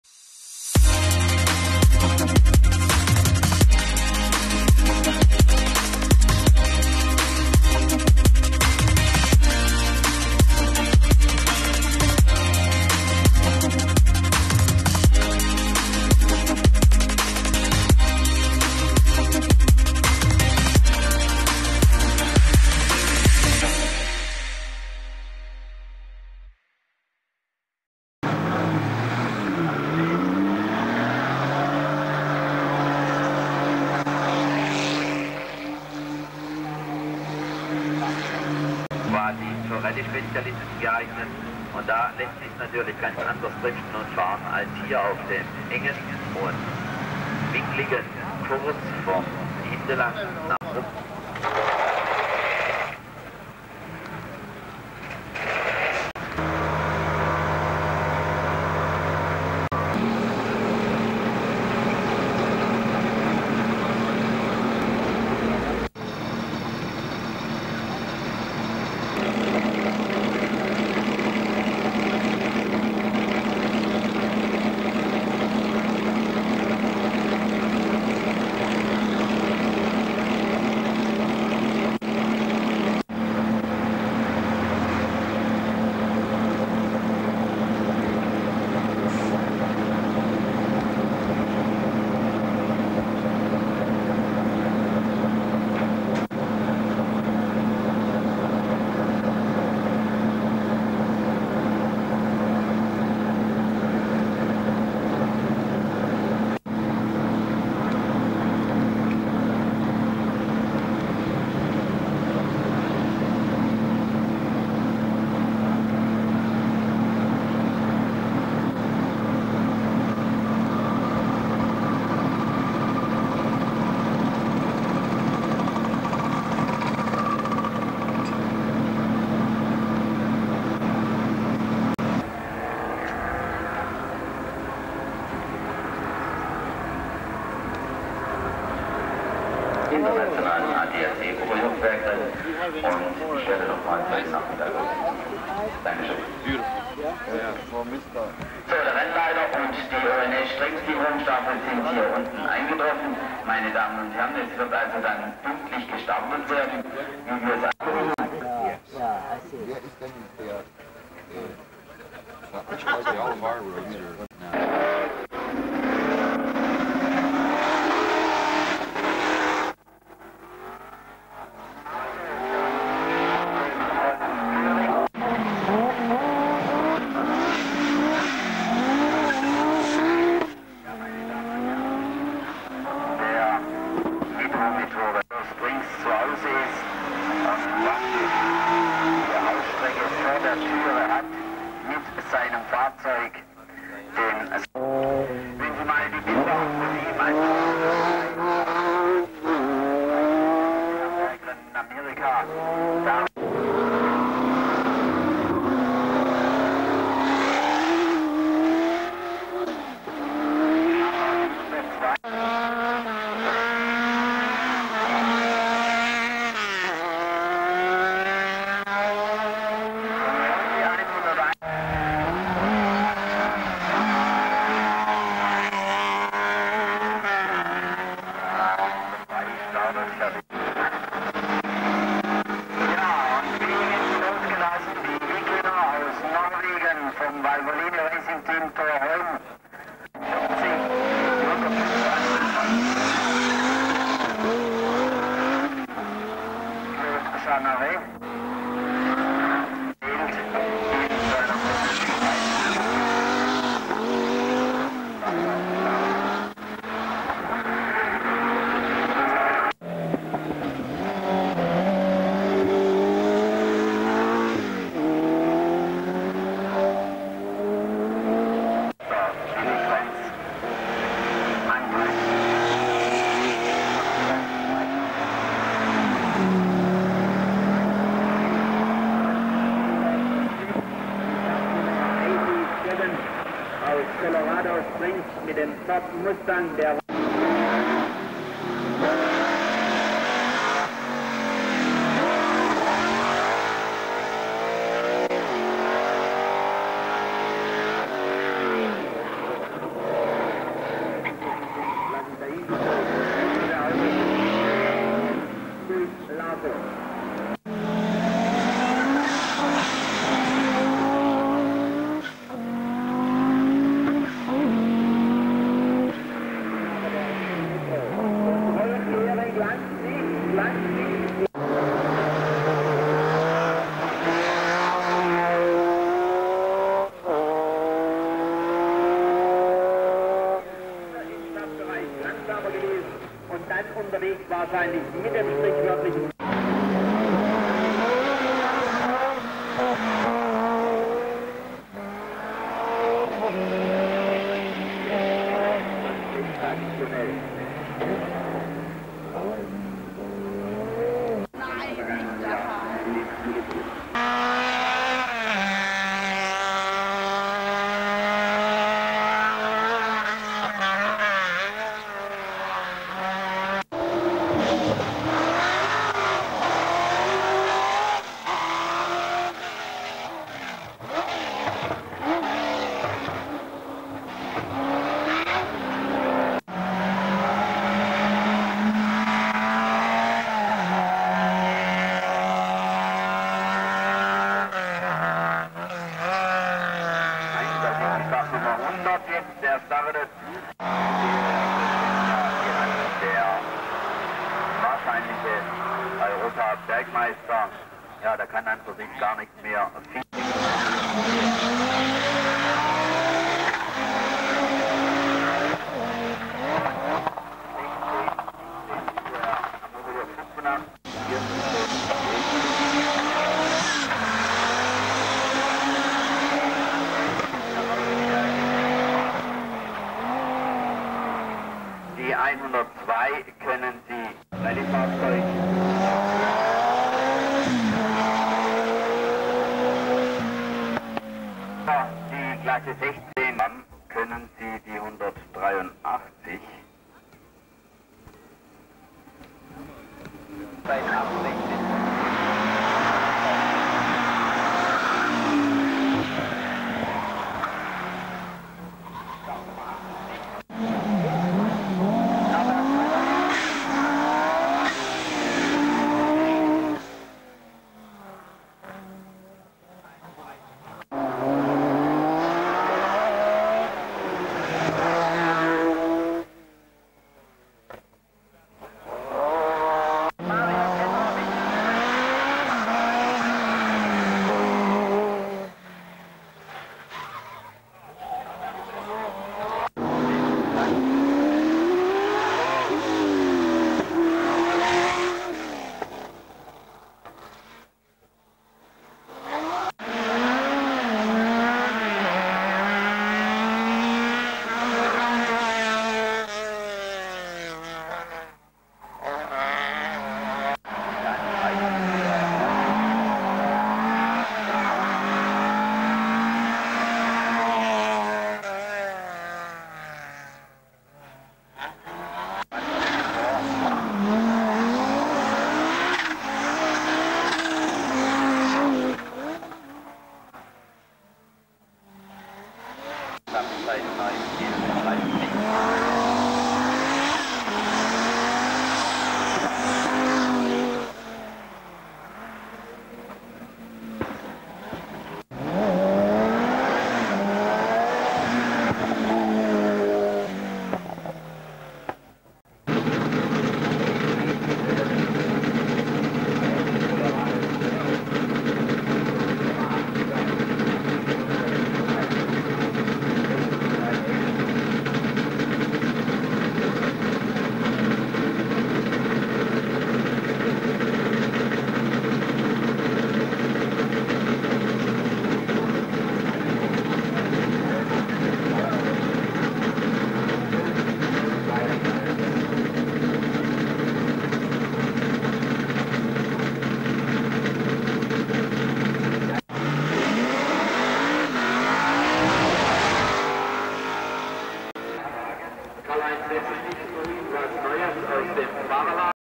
"Gruppe B Spezial" 15. Bergrennen Hindelang Oberjoch 1987 (by Bubel) Audi S1 Hillclimb Monsters
Zu der nur ein einiges Mal ausgetragenen "FiA Intercontinental Hillclimb Challenge" im Jahr 1987, zählte das Pikes Peak Bergrennen in den US und das deutsche Bergrennen Hindelang-Oberjoch. Im Allgäu sah man daher ein Spitzenfeld mit Fahrzeugen, die so nie wieder bei einem Bergrennen zu sehen waren. Zusammenfassung der Gruppe B, Open Rally, Open Wheeler, Production Rally und US-Producton.